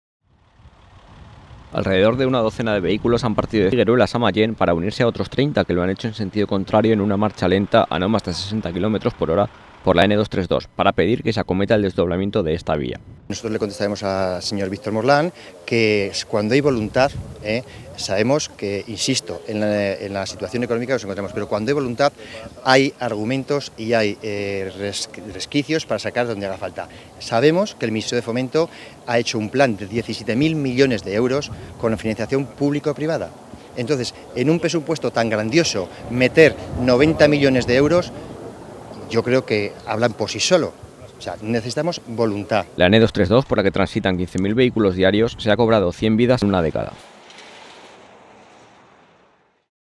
Medio centenar de vehículos reivindican por medio de una marcha lenta el desdoblamiento de la N-232
(04/02/2011) Alrededor de cincuenta vehículos han bloqueado la N-232 entre Figueruelas y Mallén circulando a la velocidad mínima permitida. El objetivo es reivindicar el desdoblamiento de esta vía, por la que transitan 15.000 vehículos y que en una década se ha cobrado un centenar de vidas.